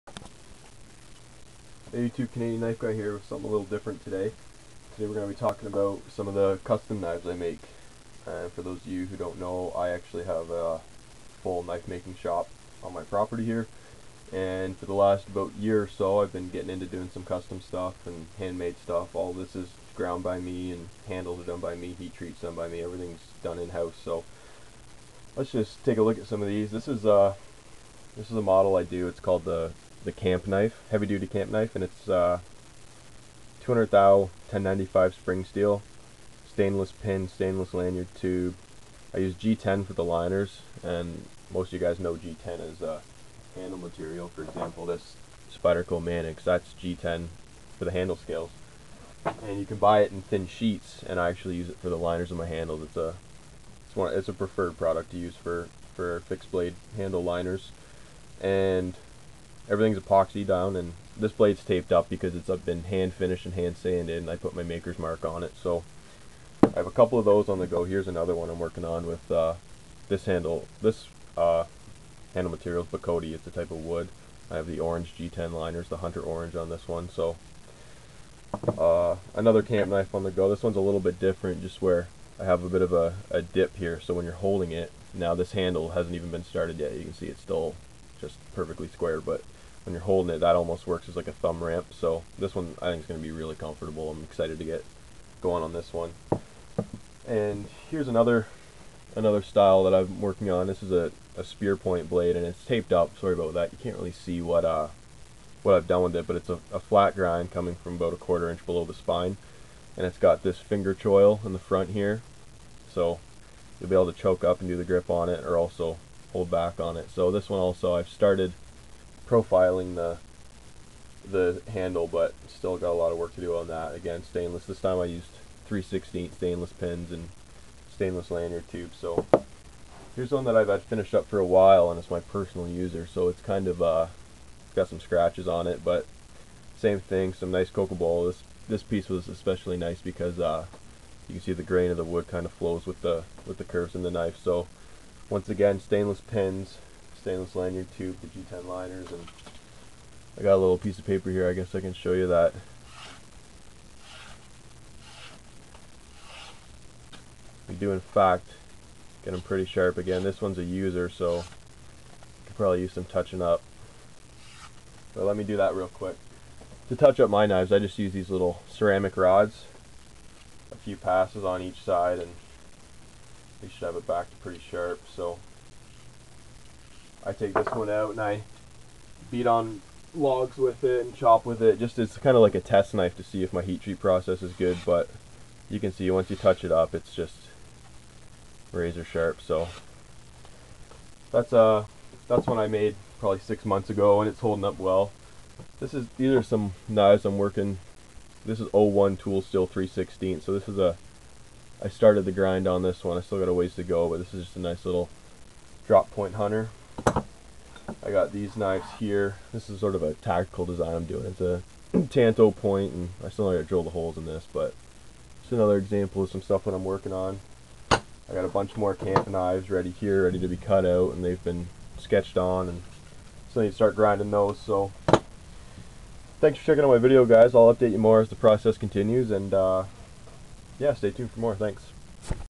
JW Knives - Custom Knives - Nov 18 11
In addition to collecting knives, I also have a complete knife making shop at home.

This is a small sample of the custom work that I do.